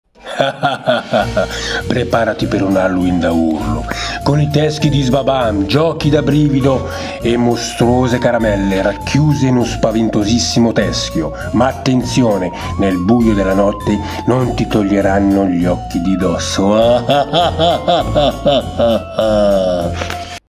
Scopri i teschi di Halloween di Sbabam!
👻 Nei negozi di giocattoli, ti aspettano i nostri spaventosissimi Teschi! Una volta aperti, troverai una bustina con un gioco a tema Halloween sbabam e un pacchetto di caramelle gommose dalle forme più mostruose 🍬👹!
Gli occhi del teschio si illuminano al buio, e poi potrai usarlo come salvadanaio o portagiochi!